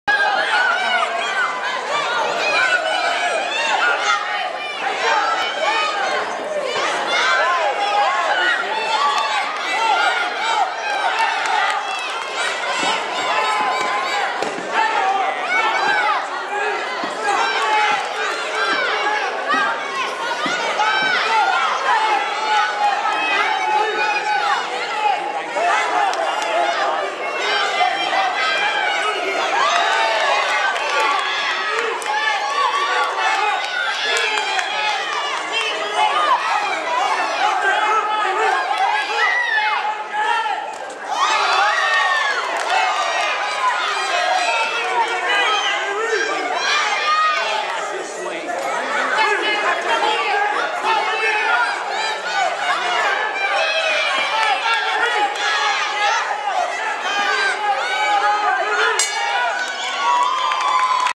Ricardo Aldaba vs Jayce Morales ( 3 of 4)
Kansas-Oklahoma Regional Golden Gloves semi-finals at the Memorial Hall  in Hutchinson, Kansas. March 26, 2010. Junior division 100 lbs contest. Ricardo Aldaba (blue trunk) of Villa Boxing club in Wichita, Kansas. Jayce Morales of Bad Boyz Boxing in Garden City, Kansas